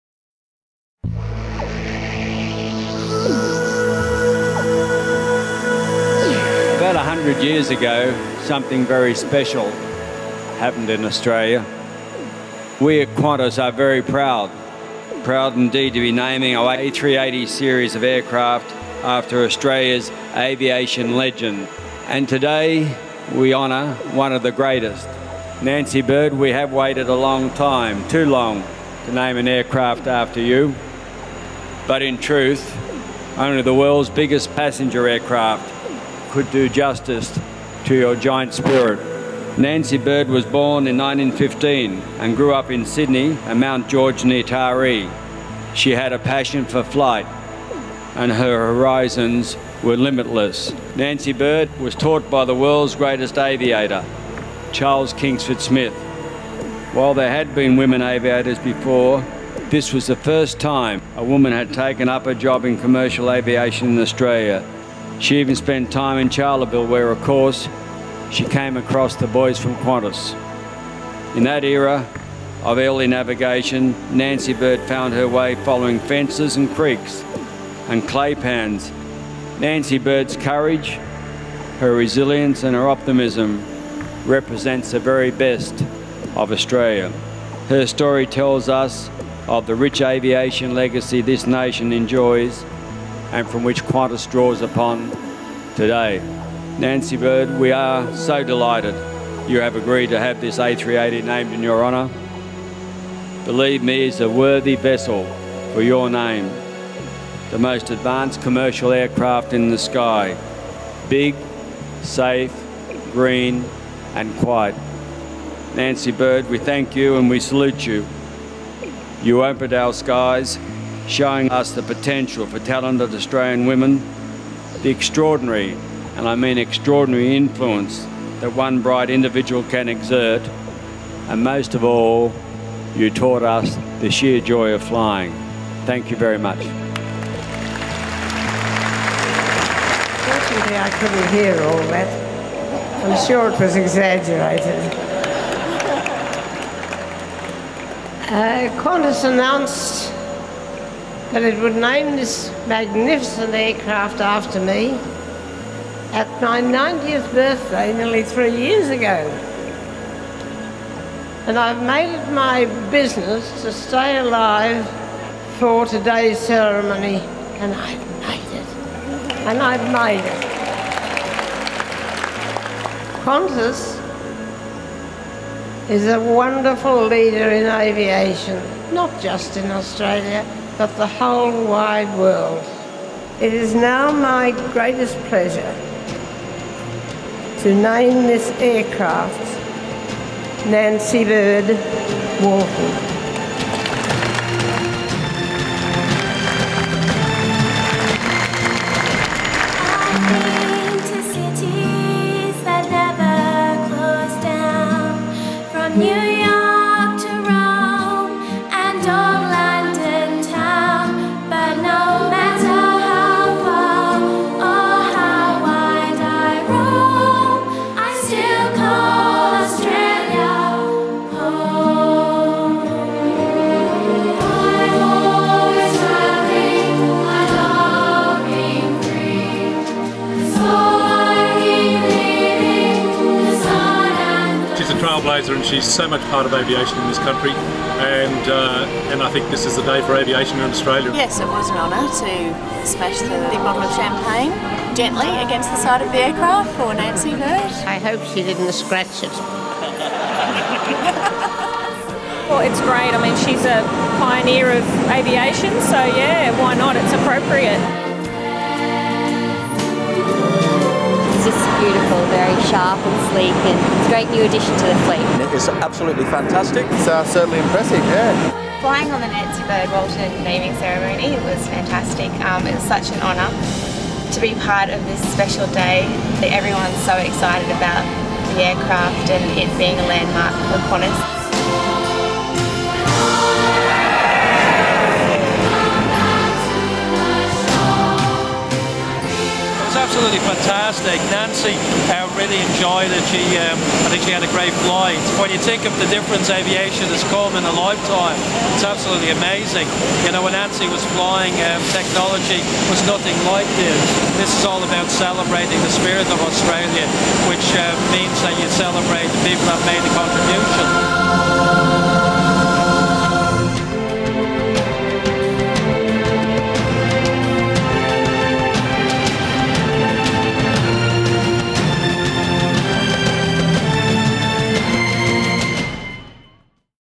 A380 Naming Ceremony - 'Nancy-Bird Walton'
Qantas honours Australian pioneer aviatrix Nancy-Bird Walton at a ceremony to officially name the airlines first A380 aircraft after her - 30 September, 2008